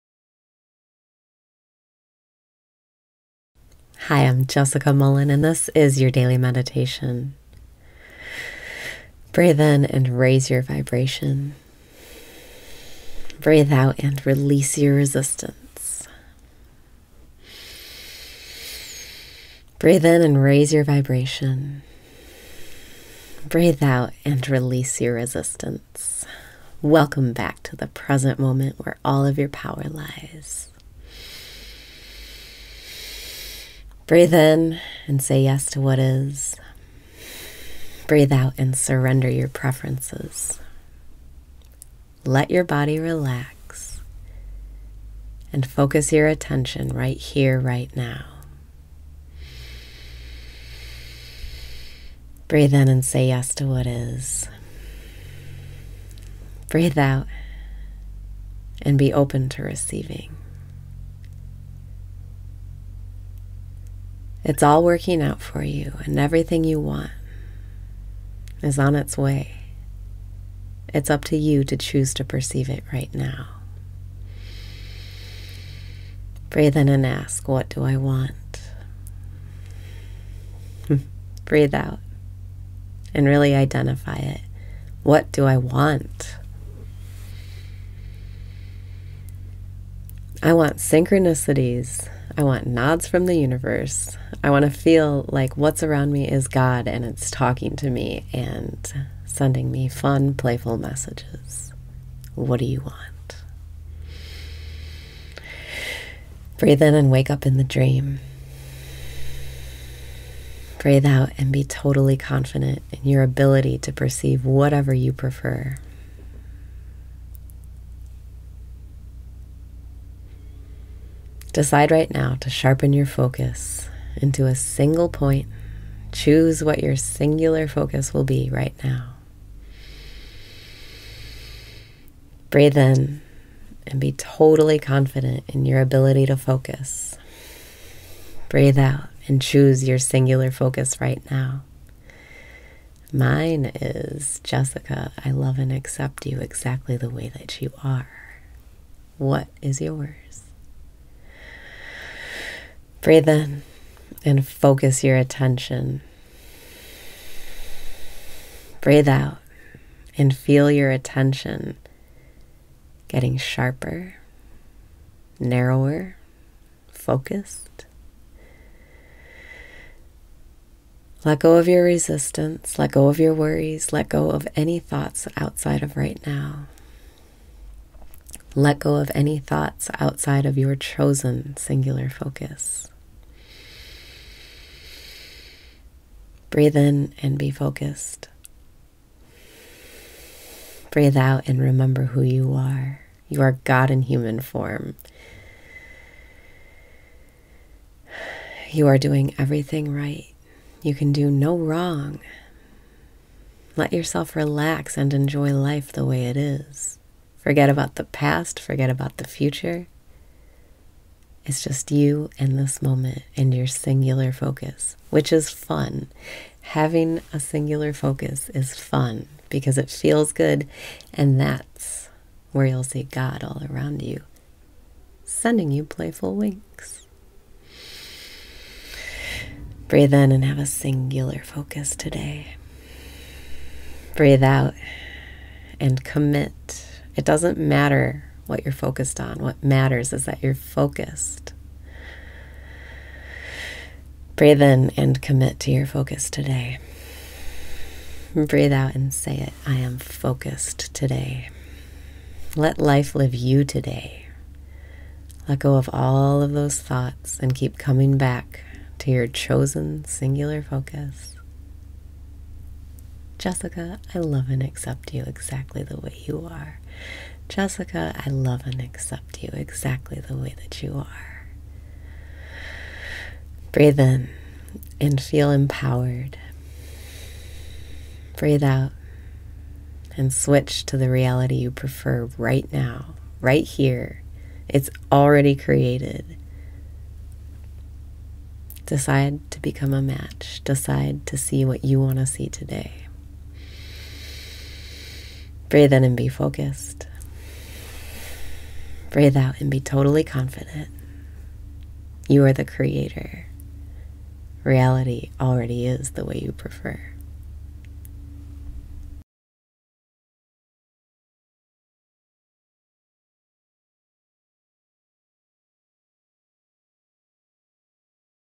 Daily Meditation - Having A Singular Focus Is Fun!
Let your body relax. And focus your attention right here, right now.

Breathe out and be open to receiving.

It’s all working out for you and everything you want is on its way. It’s up to you to choose to perceive it right now.

Breathe in and ask, “What do I want?”
Breathe out and really identify it. What do I want?

I want synchronicities. I want nods from the universe. I want to feel like what’s around me is god and it’s talking to me and sending me fun, playful messages. What do you want?

Breathe out and be totally confident in your ability to perceive whatever you prefer.

Decide right now to sharpen your focus into a single point. Choose what your singular focus will be, right now.

Breathe in and be totally confident in your ability to focus.
Breathe out and choose your singular focus right now.

Mine is, “Jessica, I love and accept you exactly the way you are.” What is yours?

Breathe out and feel your attention getting sharper, narrower, focused.

Let go of your resistance. Let go of your worries. Let go of any thought outside of right now. Let go of any thoughts outside of your chosen singular focus.

Breathe in and be focused.
Breathe out and remember who you are.

You are god in human form. You are doing everything right. You can do no wrong. Let yourself relax and enjoy life the way it is. Forget about the past, forget about the future. It’s just you and this moment and your singular focus. Which is fun! Having a singular focus is fun! Because it feels good and that’s where you’ll see god all around you, sending you playful winks!

Breathe in and have a singular focus today!
Breathe out and commit.

It doesn’t matter what you’re focused on. What matters is that you’re focused.

Breathe in and commit to your focus today.
Breathe out and say it: I am focused today!

Let life live you today. Let go of all of those thoughts and keep coming back to your chosen singular focus. “Jessica, I love and accept you exactly the way you are. Jessica, I love and accept you exactly the way that you are.”

Breathe in and feel empowered!
Breathe out and switch to the reality you prefer, right now, right here. It’s already created. Decide to become a match. Decide to see what you want to see today.

Breathe out and be totally confident. You are the creator. Reality already is the way you prefer.